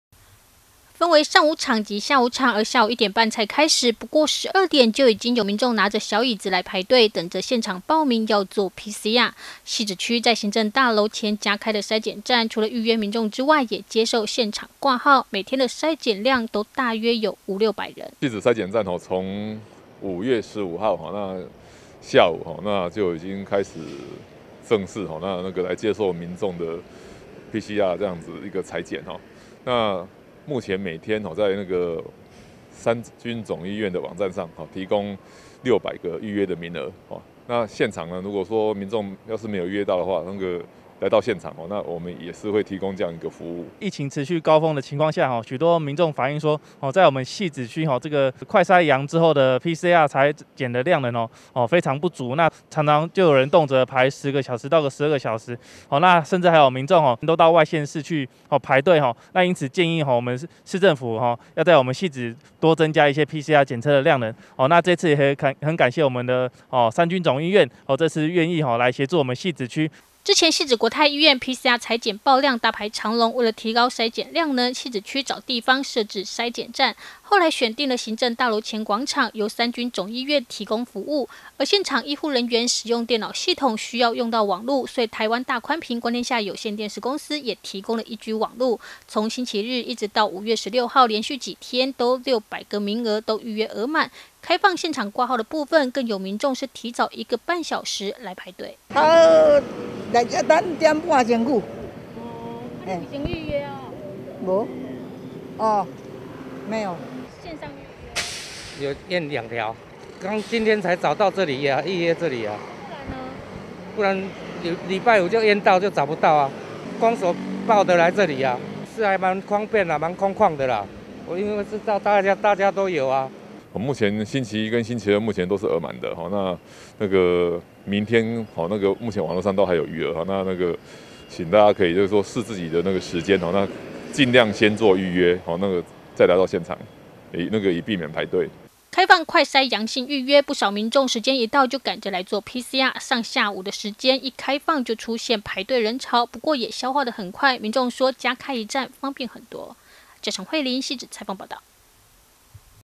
PCR篩檢加開公所篩檢站 民眾：方便很多
為了提高PCR篩檢量能，汐止區行政大樓前加開篩檢站，由三軍總醫院提供服務，每天接受預約六百人，這幾天都額滿，不過，也有不少民眾爽約，所以，篩檢站也接受現場報名，就有民眾提早一個半小時來排隊，現場掛號要做pcr。